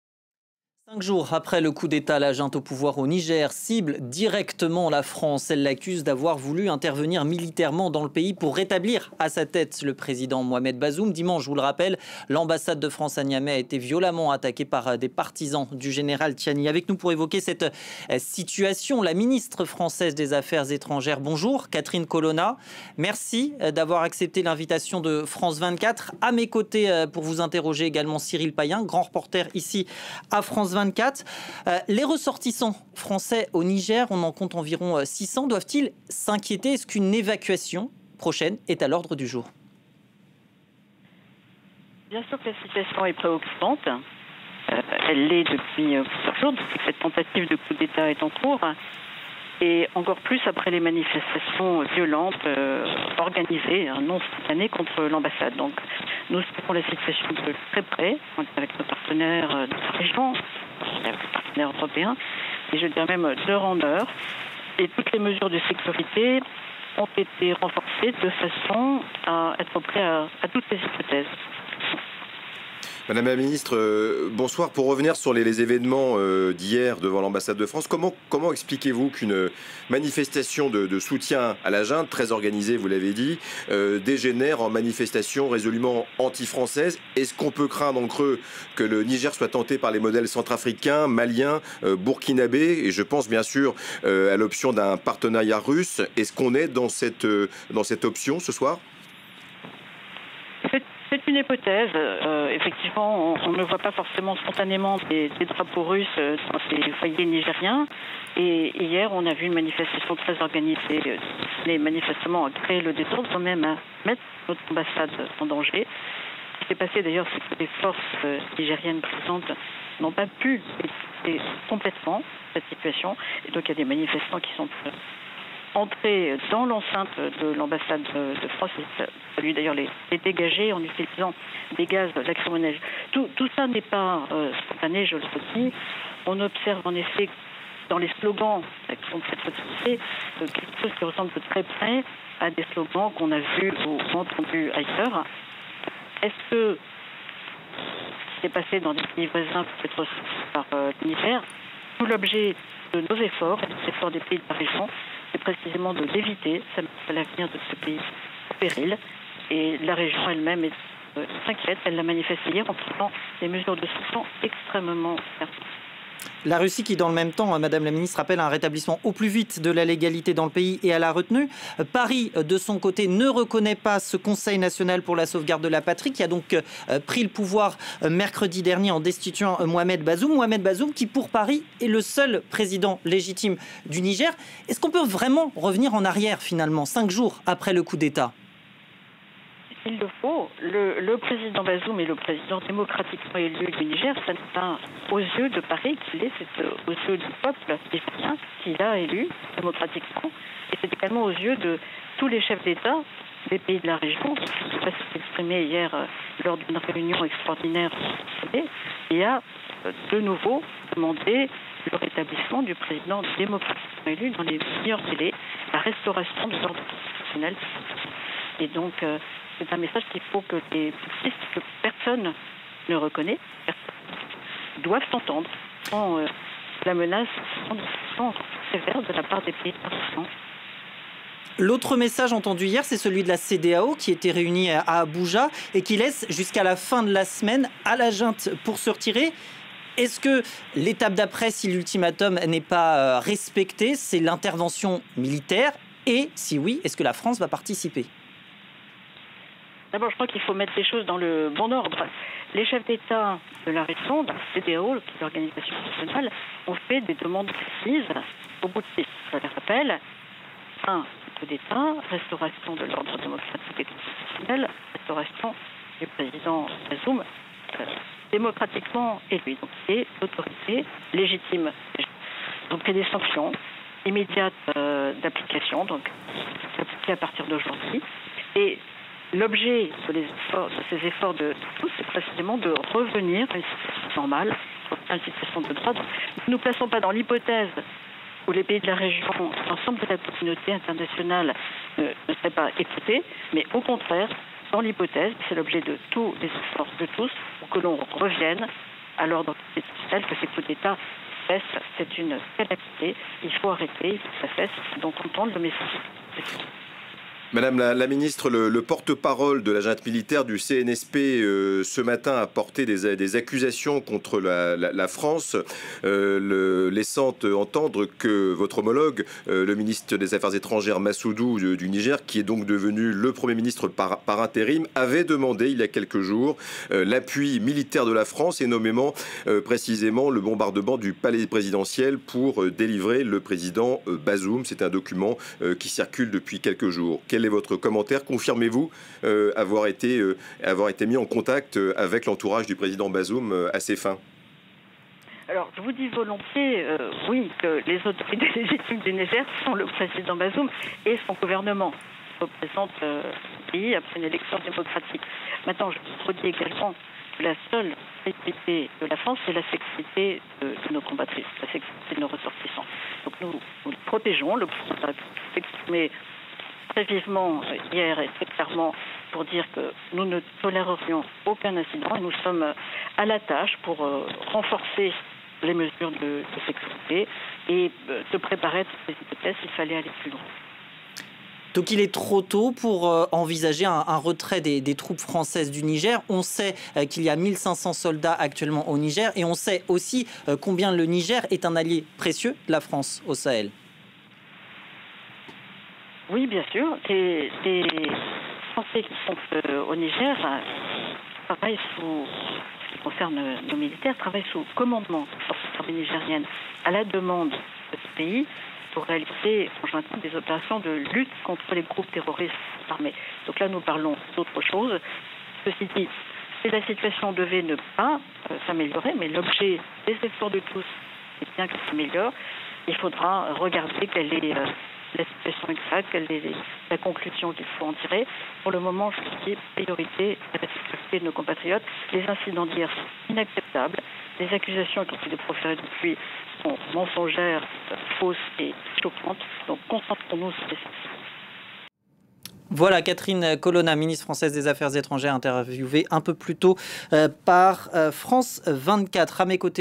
Pour Catherine Colonna, "la seule priorité de la France, c’est la sécurité de nos ressortissants"
Cinq jours après le coup d'État au Niger, la cheffe du Quai d'Orsay, Catherine Colonna, a réaffirmé à France 24 que la sécurité des ressortissants français reste la seule priorité de la France.